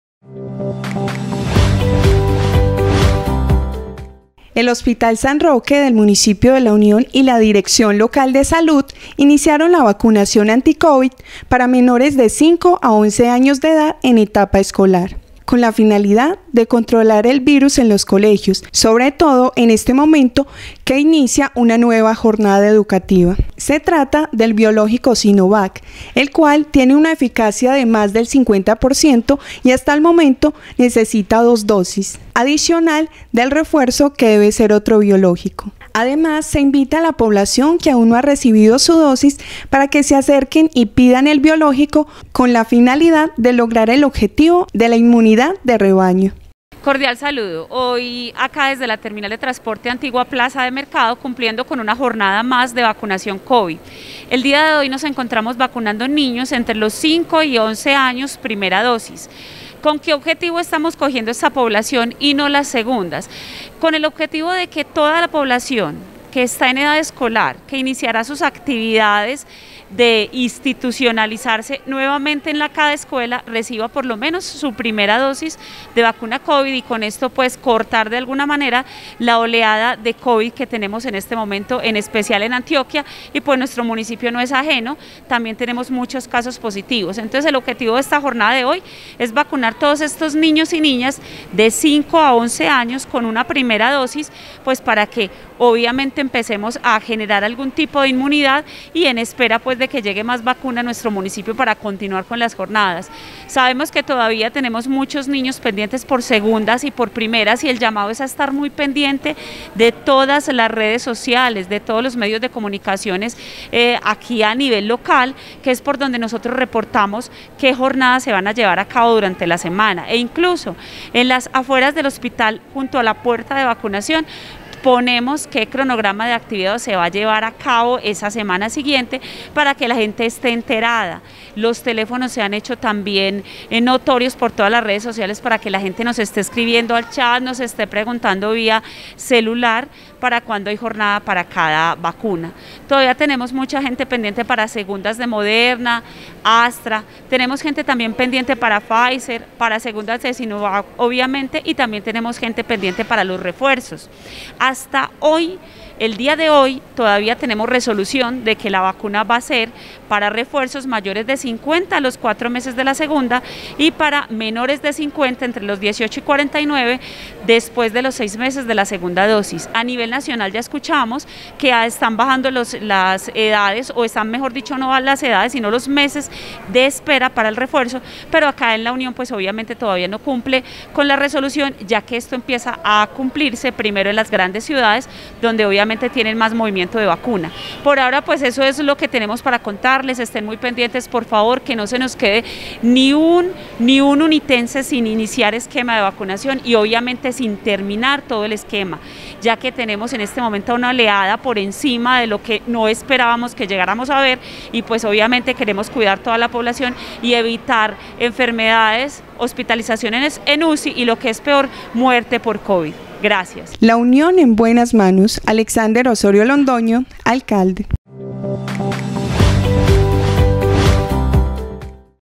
VACUNACIÓN ANTICOVID 19 PARA MENORES DE 5 A 11 AÑOS
El Hospital San Roque, del municipio de La Unión y la Dirección Local de Salud iniciaron con la vacunación anticovid para menores de 5 a 11 años de edad en etapa escolar, con la finalidad de controlar el virus en los colegios, sobre todo en este momento que inicia una nueva jornada educativa.

Se trata del biológico Sinovac, el cual tiene una eficacia de más del 50 % y que hasta el momento necesita dos dosis, adicional del refuerzo que debe ser de otro biológico.

Además, se invita a la población que aún no ha recibido su dosis, para que se acerquen y pidan el biológico, con la finalidad de lograr el objetivo de la inmunidad de rebaño.